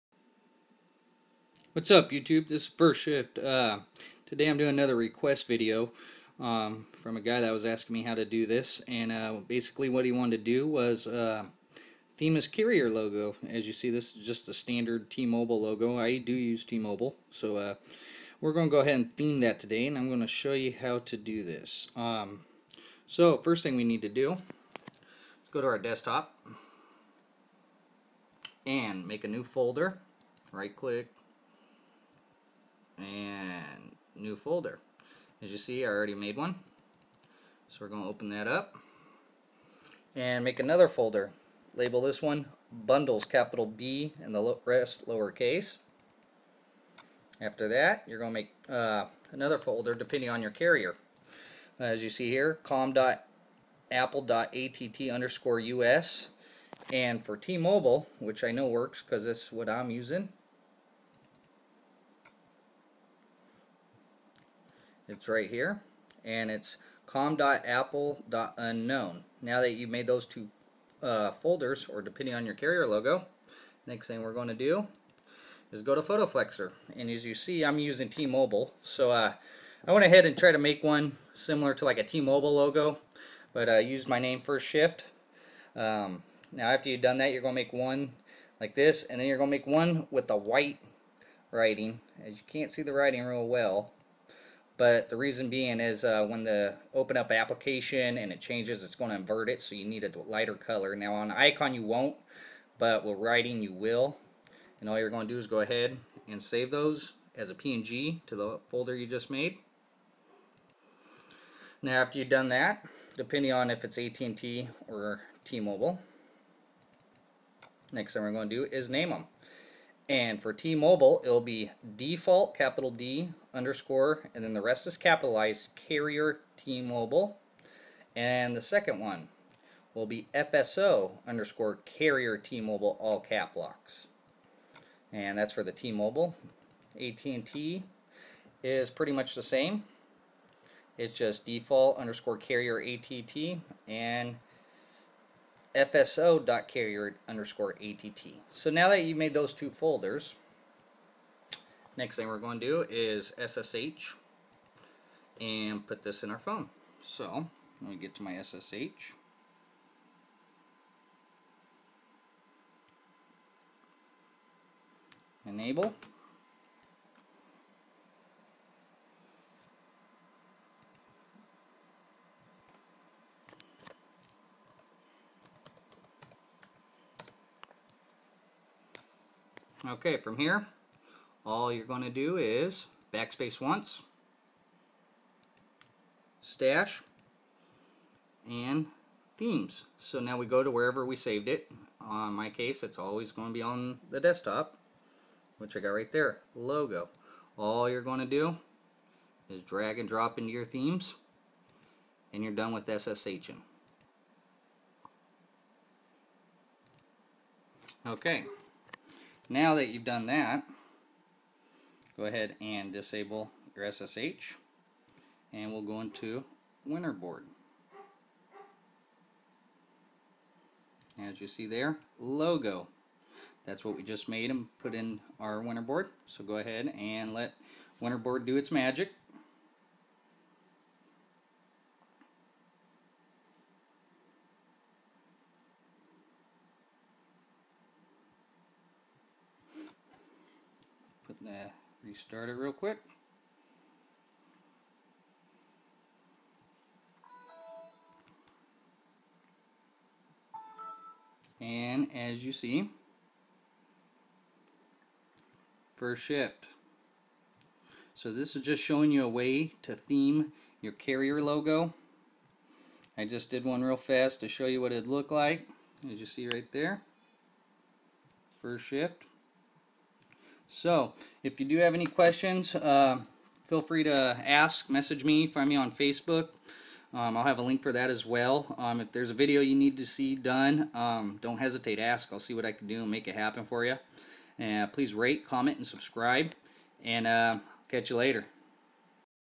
Make custom Carrier logo on 3.0
This vid will show you how to Theme your carrier logo.

1.You will make a folder named what you like.

2.Make a folder inside of the one you just made named Bundles.

3.Make one more folder named com.apple.ATT_US for ATT users and com.apple.Unknown for T MOBILE users.

4.Inside that folder is where you will save your icons.The first one is named: Default_CARRIER_TMOBILE second one named FSO_CARRIER_TMOBILE for TMOBILE users.
ATT users name them Default_CARRIER_ATT , FSO_CARRIER_ATT

5. SSH your theme into WinterBoard and enjoy your themed logo !